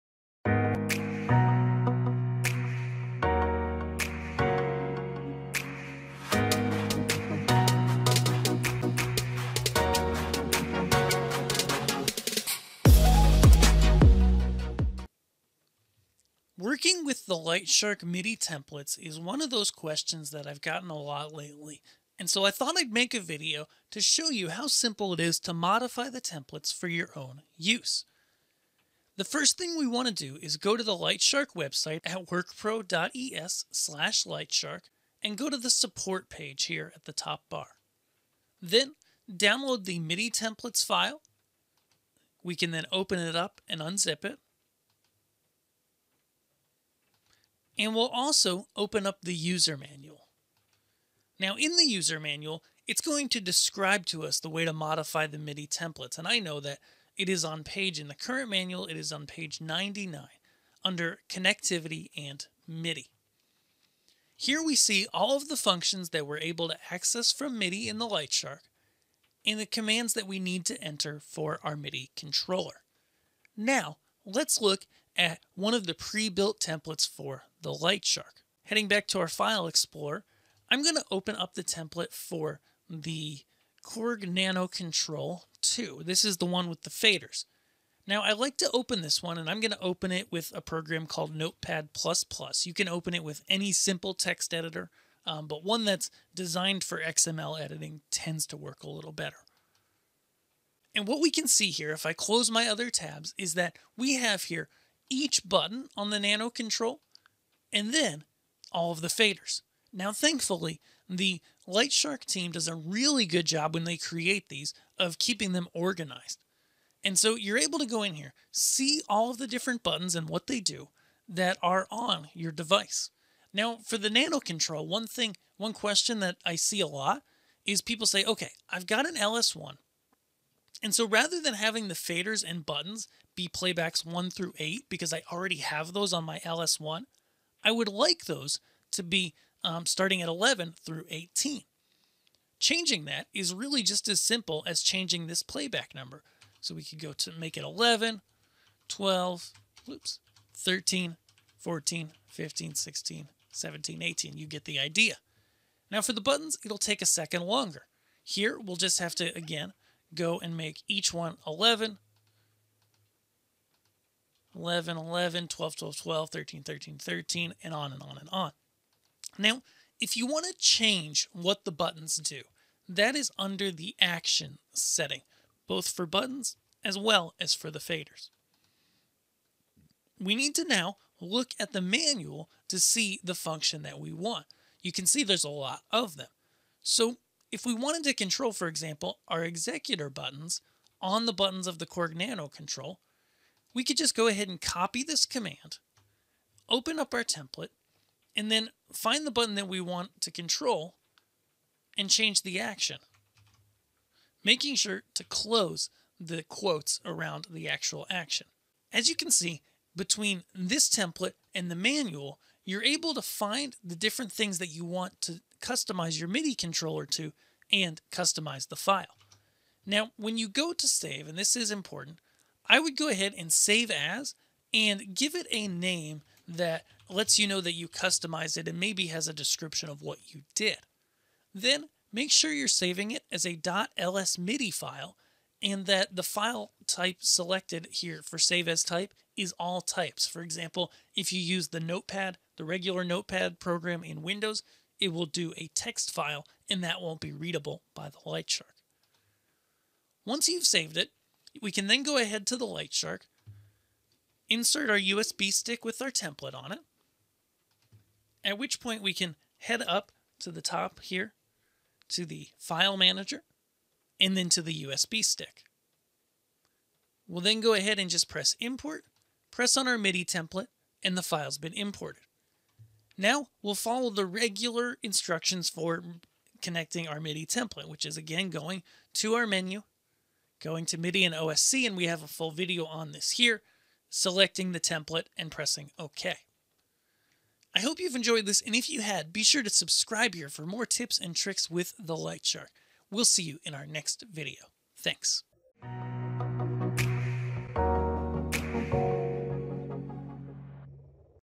Editing LightShark MIDI Templates
Want to customize the lightShark MIDI templates?  Here's how - it's easier than you think!